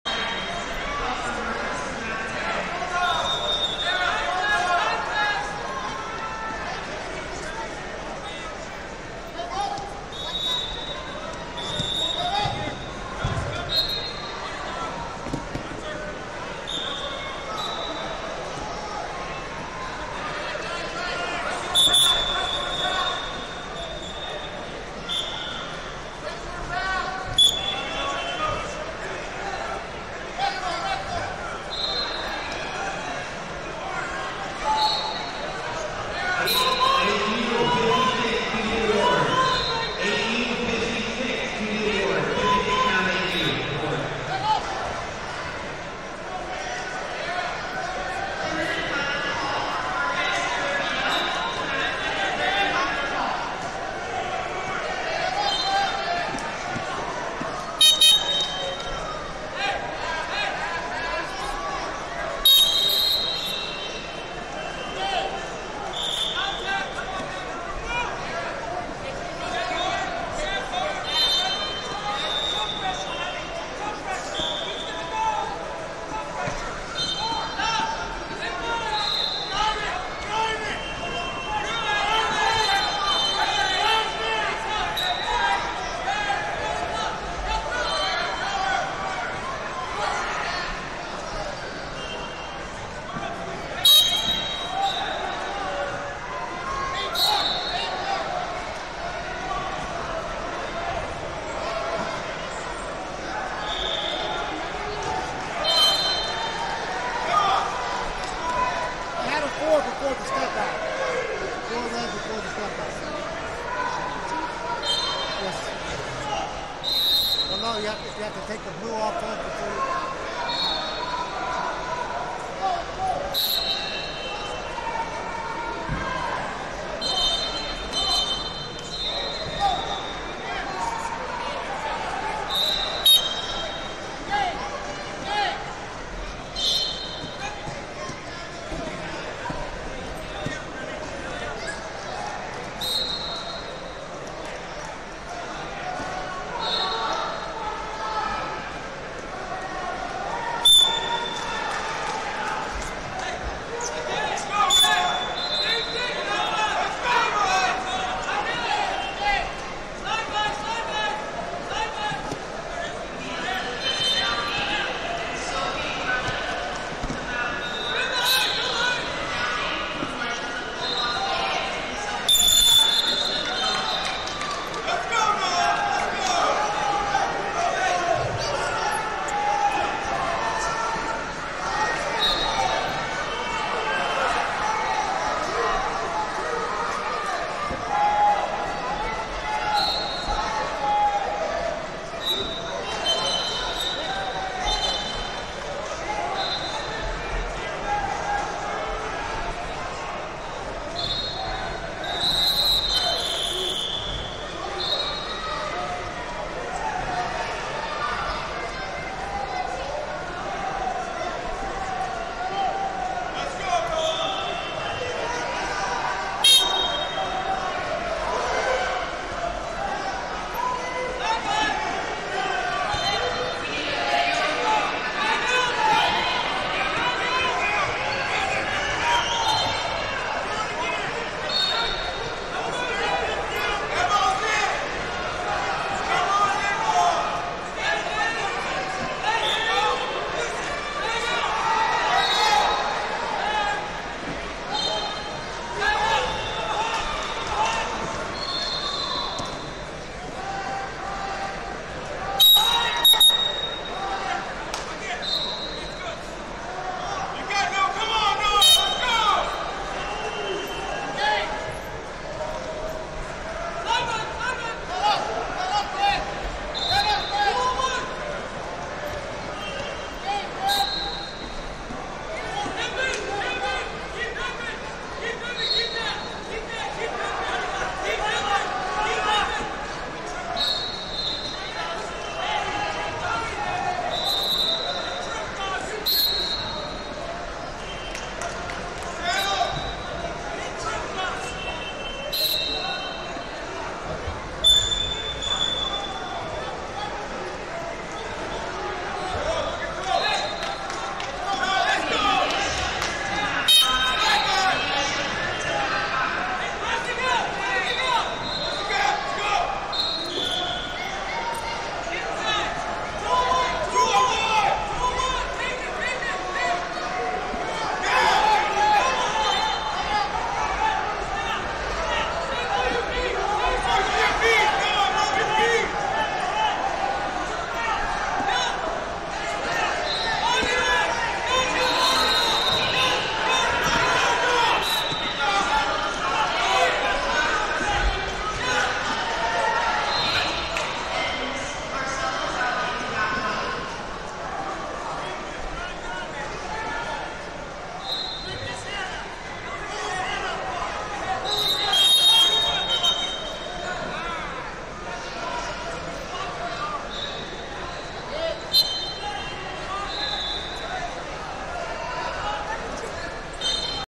12U BOYS 74 Remington Grunhuvd Silver Bow Amateur Wrestling Assoc. Vs Noah Moylan Poway Wrestling
2021 Western Regionals GR - 12U BOYS 74 Remington Grunhuvd Silver Bow Amateur Wrestling Assoc. Vs Noah Moylan Poway Wrestling